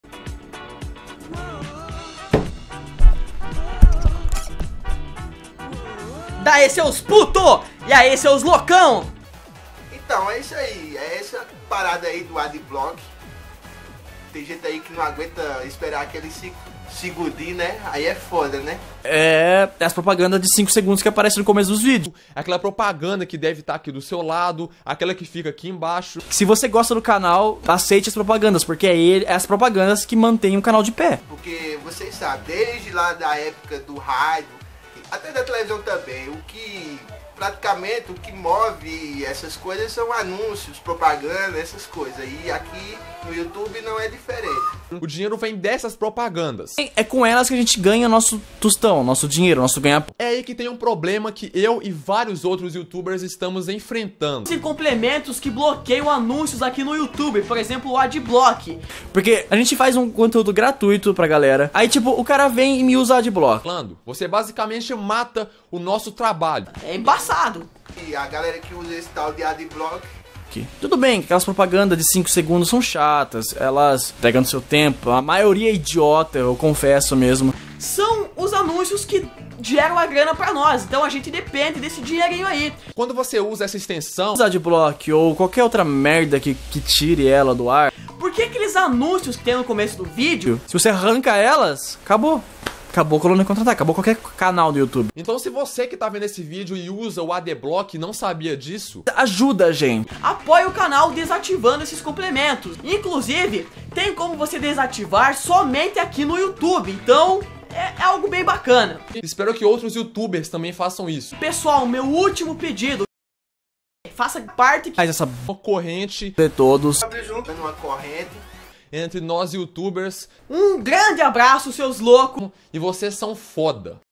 MENSAGEM SOBRE ADBLOCK - DIGA NÃO AO ADBLOCK
GambiaHero Gamecube PS3FAT ArlessonSilva ConsertoDoInscrito ArliissonxD Venho por meio desse vídeo , Passar a Minha mensagem a respeito do Adblock , embora seja uma Algo que tira as Propaganda do youtube , tira Nosso Dinheirinho também , e esse Dinheirinho Ajuda Muito , Tanto para comprar equipamentos pro Canal , quanto pra Sobreviver também ... Espero que esse vídeo Ajude Nossos Amigos dos quais eu Curto muito o Trabalho deles ..

Vamos Nós Unir nessa Corrente , Não Usem o Adblock :D

Agradeço pela Iniciativa ao Sr. Wilson do Colônia Contra Ataca ,
e aos Demais Parceiros de Jogatina e Bons Conteúdos que São

Gusang - Assopra Fitas
Rafael - Vídeos de Clássicos
e ao Nosso Amigo Fabiano do Canal - Games Veios